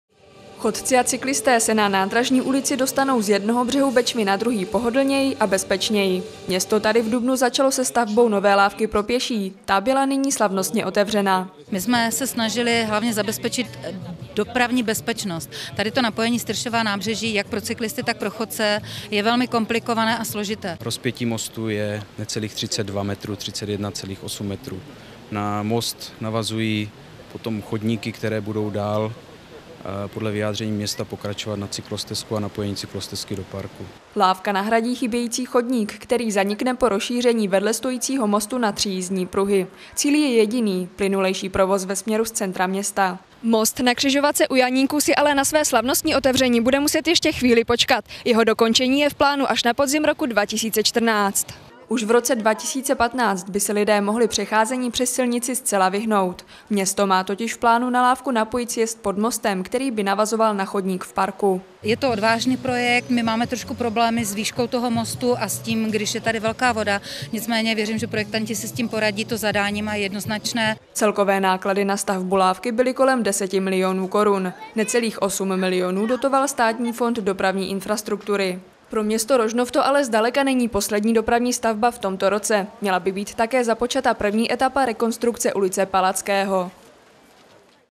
Po nové lávce přes rožnovskou Bečvu se prošli první chodci
V Rožnově pod Radhoštěm byla slavnostně otevřena nová lávka pro pěší přes řeku Bečvu. Zbudována byla hlavně z důvodů rozšiřování mostu v blízkosti křižovatky U Janíků na tři jízdní pruhy. Chodci a cyklisté se tak přecházení přes most můžou zcela vyhnout.